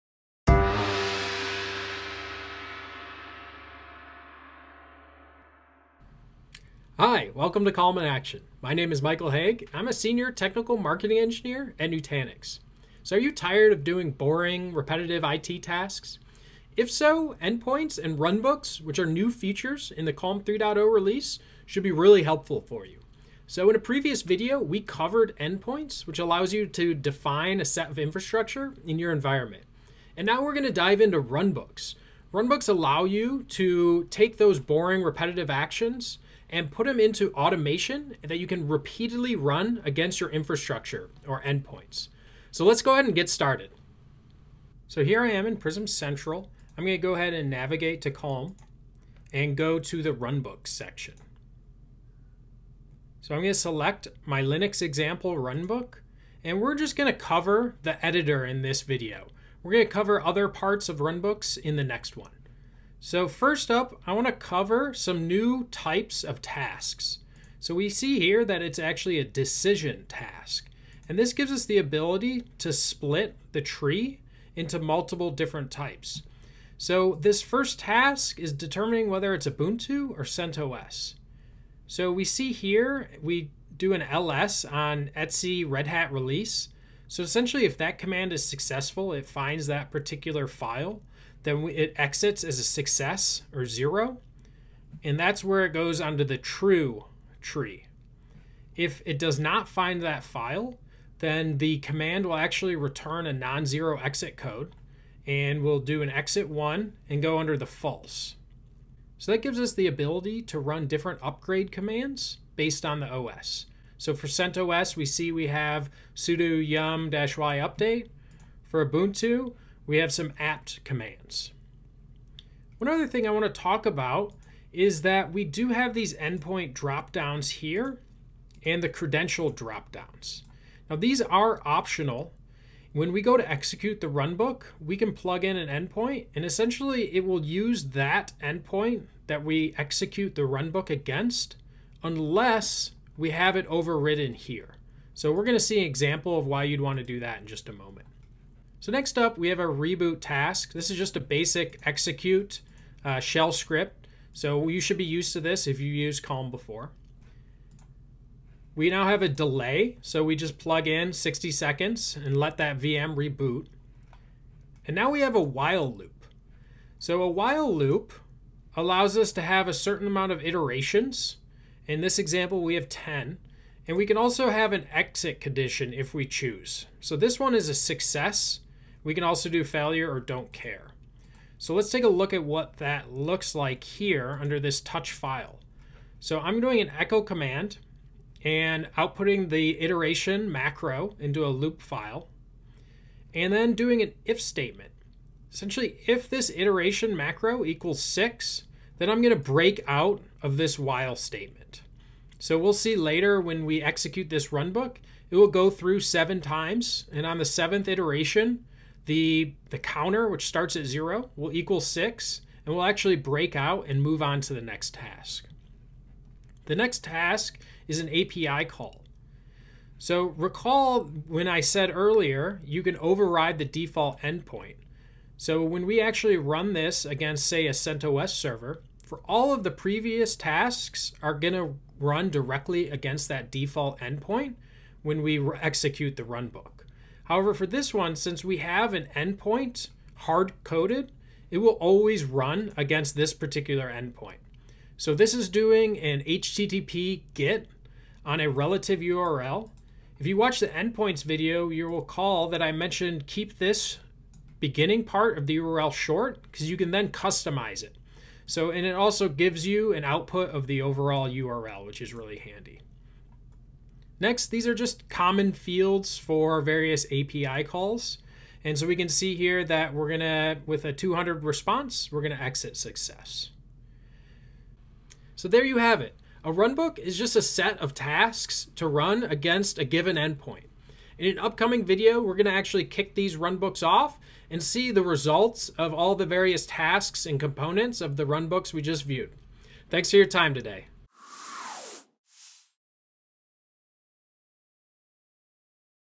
Calm in Action: Runbooks: Editor
Tired of doing repetitive IT tasks? In this video, Michael Haigh walks you through the Runbooks editor in Nutanix Calm to help turn repetitive, manual tasks into easy, consistent operations.